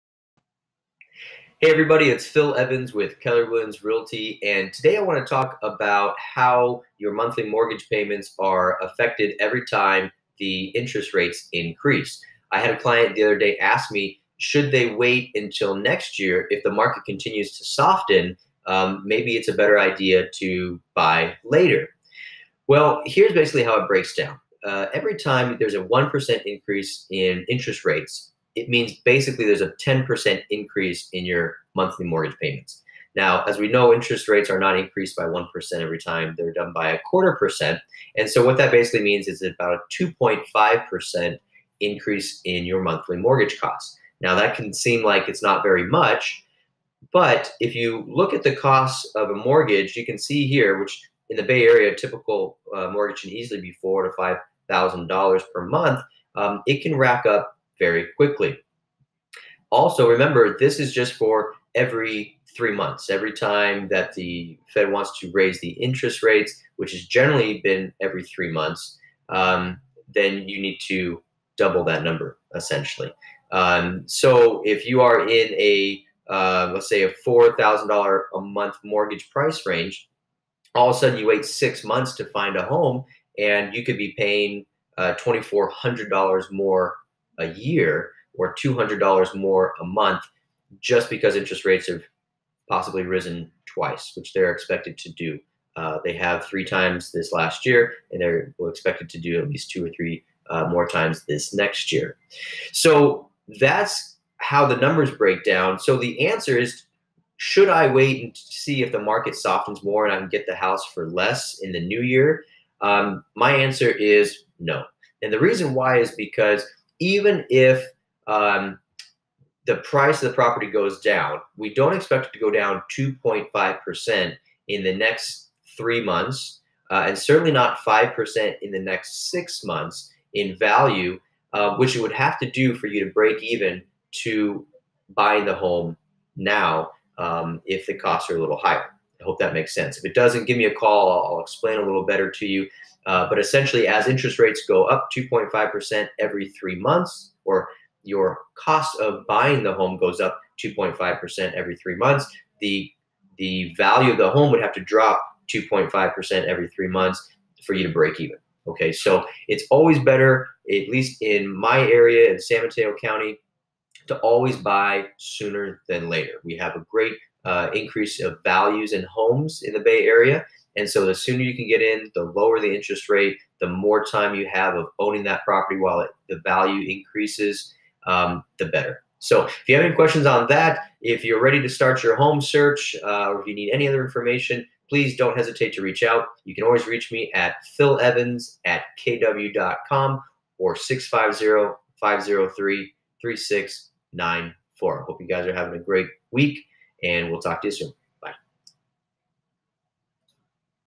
How Interest Rates Effect Your Mortgage Payments
Realtor Phil Evans talks shows how increases in interest rates effect your monthly mortgage payments. With interest rates increasing every three months generally, deciding whether you should wait to buy is a big decision. Phil Evans walks you though the pros and cons. If you have any questions please reach out in comments.